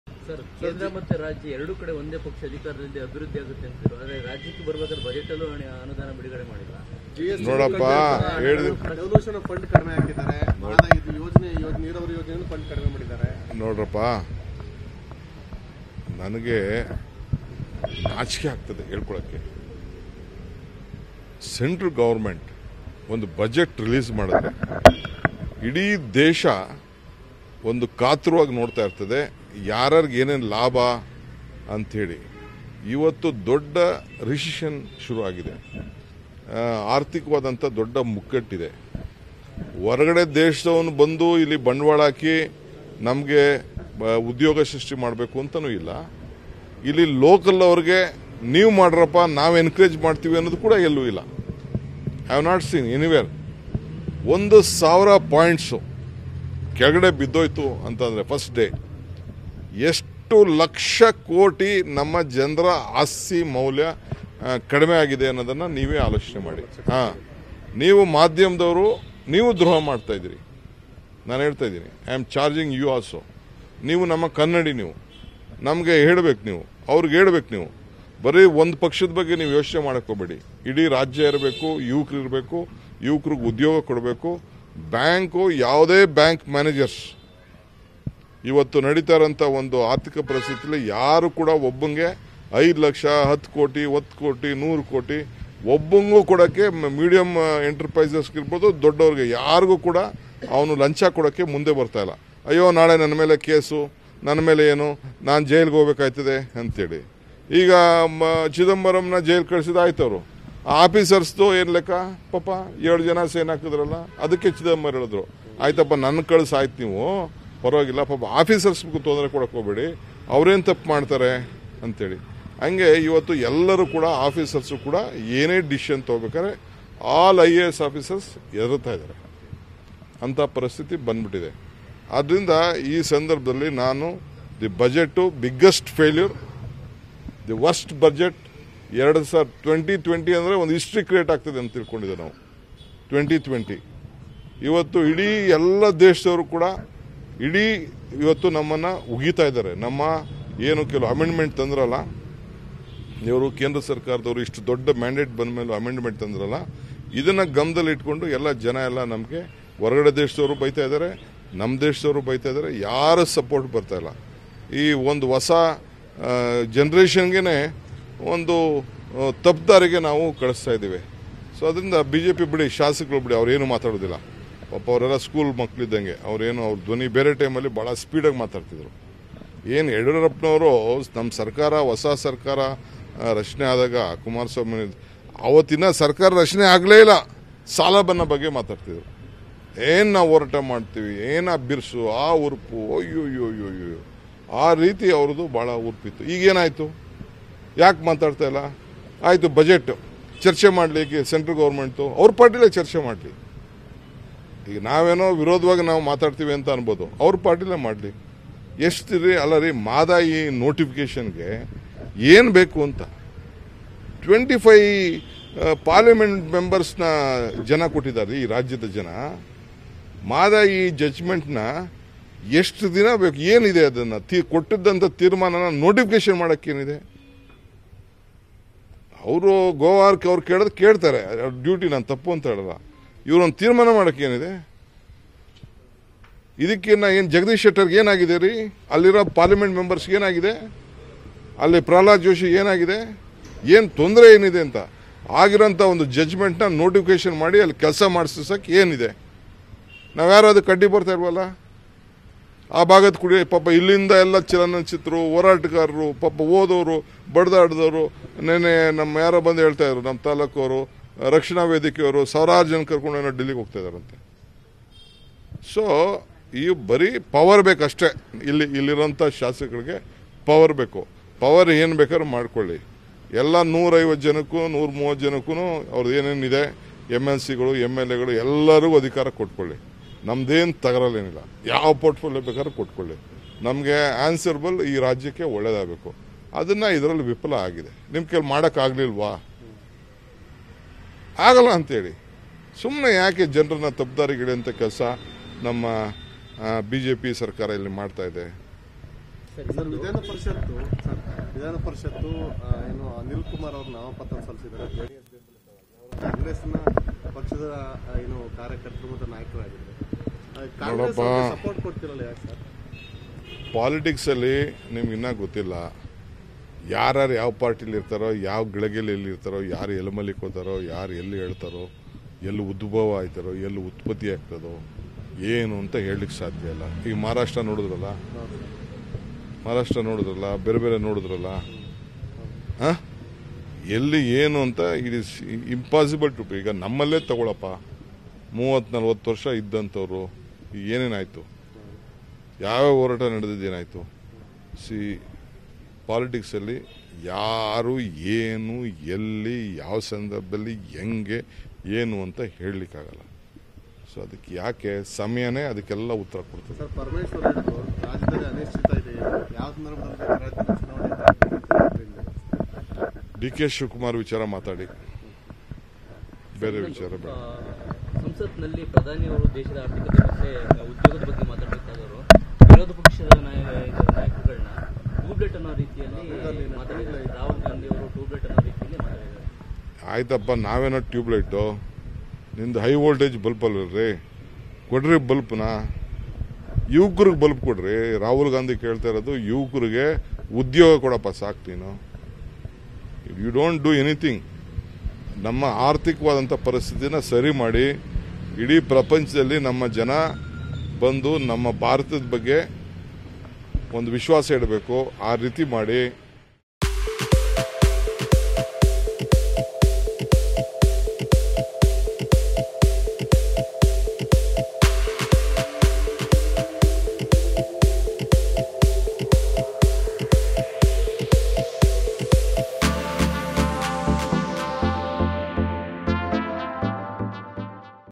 ಕೇಂದ್ರದ ಬಜೆಟ್ಟು ಬಿಗ್ಗೆಸ್ಟ್ ಫೇಲ್ಯೂರು, ವರ್ಸ್ಟ್ ಬಜೆಟ್: DK Shivakumar
ಬೆಂಗಳೂರು(ಫೆ.08): ಕಾಂಗ್ರೆಸ್​ ನಾಯಕ ರಾಹುಲ್​ ಗಾಂಧಿಯನ್ನು ಟ್ಯೂಬ್​ಲೈಟ್​ ಎಂದು ಕರೆದು ವ್ಯಂಗ್ಯ ಮಾಡಿದ್ದ ಪ್ರಧಾನಿ ಮೋದಿಗೆ ಮಾಜಿ ಸಚಿವ ಡಿಕೆ ಶಿವಕುಮಾರ್​ ತಿರುಗೇಟು ನೀಡಿದ್ಧಾರೆ.ಇಂದು ಬೆಂಗಳೂರಿನಲ್ಲಿ ಸುದ್ದಿಗಾರರೊಂದಿಗೆ ಮಾತನಾಡಿದ ಅವರು, "ನಾವೇನೋ ಟ್ಯೂಬ್​ಲೈಟ್​, ನಿಮ್ಮದು ಹೈ ವೋಲ್ಟೇಜ್​ ಅಲ್ವಾ? ಹಾಗಿದ್ರೆ ಮೊದಲು ಯುವಕರಿಗೆ ಬಲ್ಭ್​ ಕೊಡಿ, ಎಲ್ಲರಿಗೂ ಉದ್ಯೋಗ ಕೊಡ್ರಪ್ಪಾ.. ನಮ್ಮ ಆರ್ಥಿಕತೆಯನ್ನು ಬೇರೆ ದೇಶದವರು ನೋಡಿ ಹೊಗಳಬೇಕು. ಆ ಕೆಲಸ ಮೊದಲು ಮಾಡ್ರಪ್ಪಾ," ಎಂದು ಪ್ರಧಾನಿ ಮೋದಿಗೆ  ತಿರುಗೇಟು ಕೊಟ್ಟಿದ್ದಾರೆ.